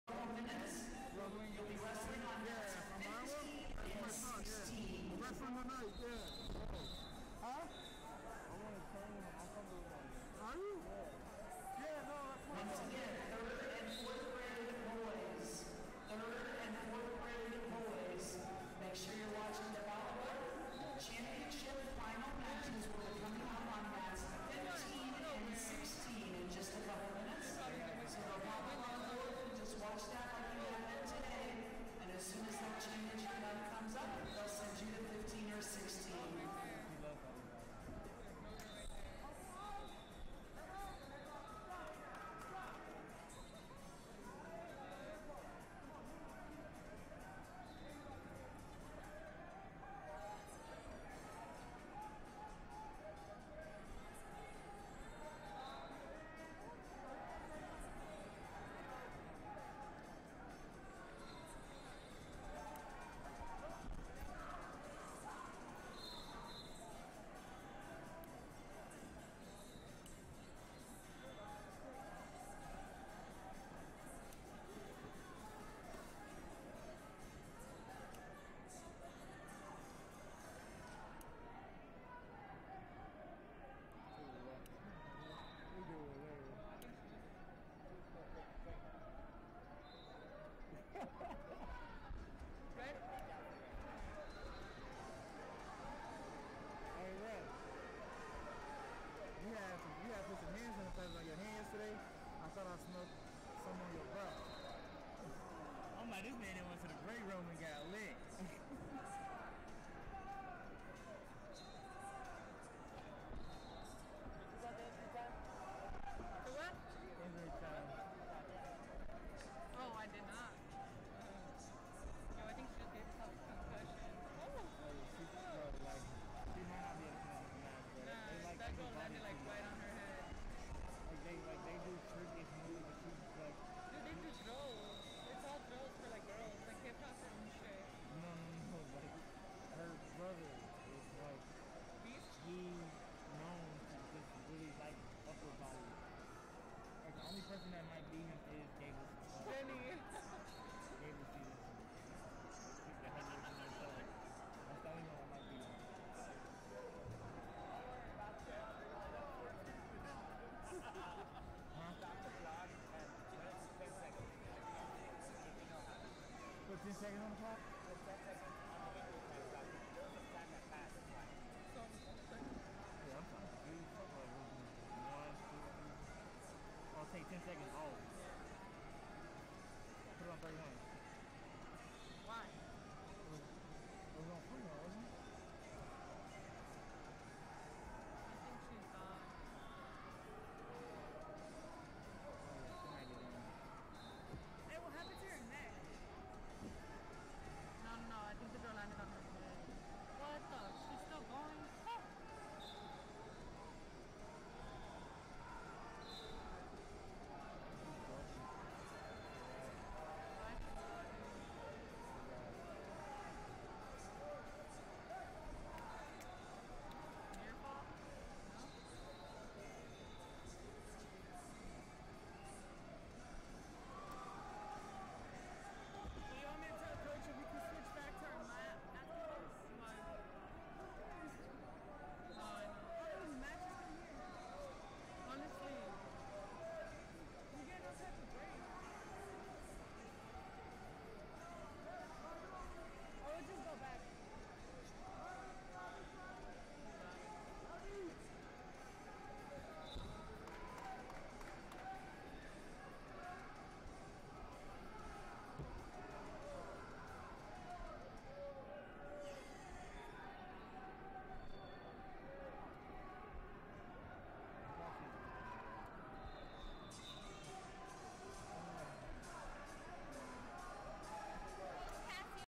2020 USAW Preseason Nationals/5274 High School Girls 152 Avery Beckman Vici Wrestling Club Vs Rose
2020 USAW Preseason Nationals - 2020 USAW Preseason Nationals/5274 High School Girls 152 Avery Beckman Vici Wrestling Club Vs Rose Cassioppi Hononegah Wrestling Club